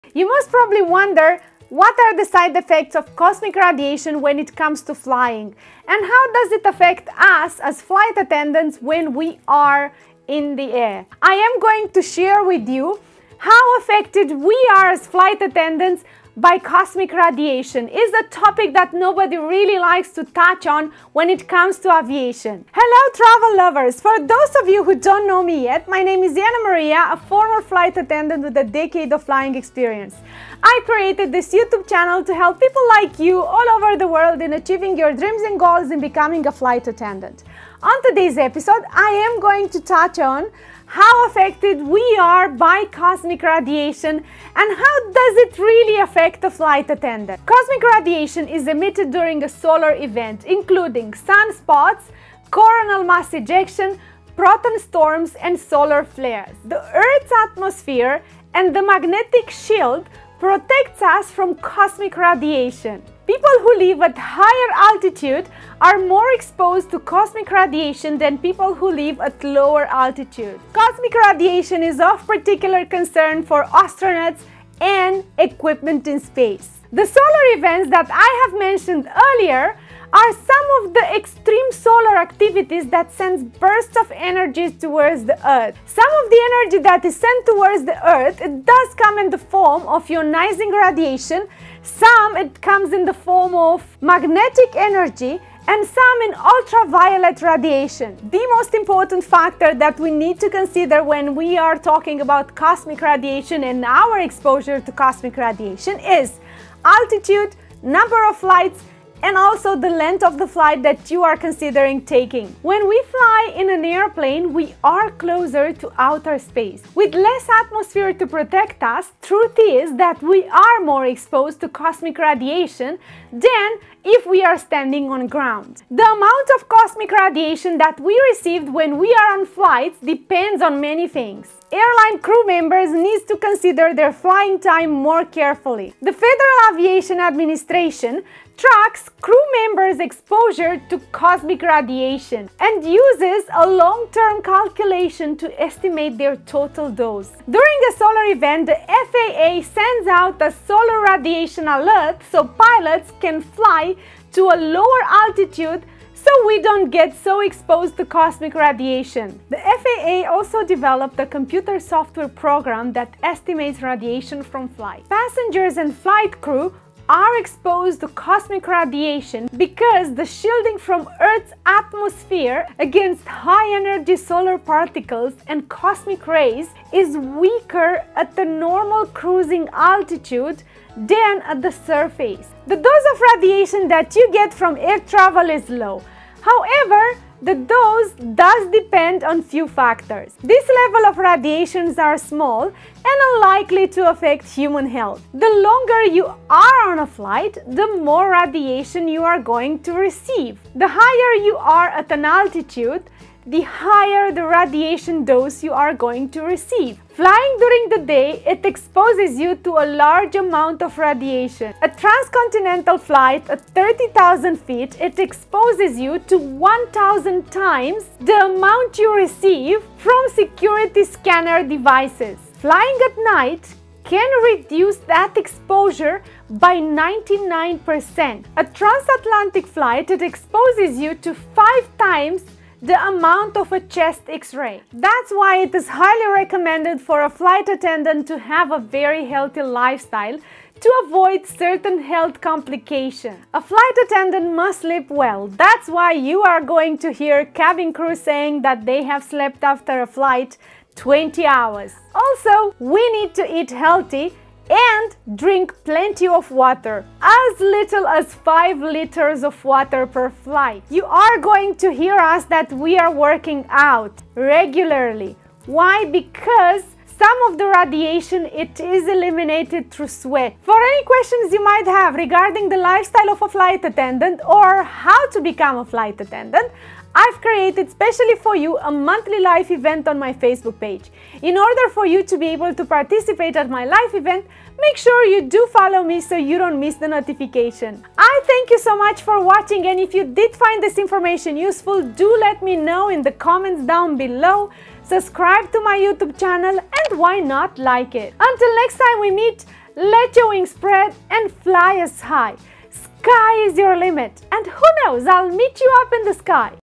Radiation from Air Travel|How does Cosmic Radiation affect Flight Attendants
We are exposed to low levels of radiation when we fly.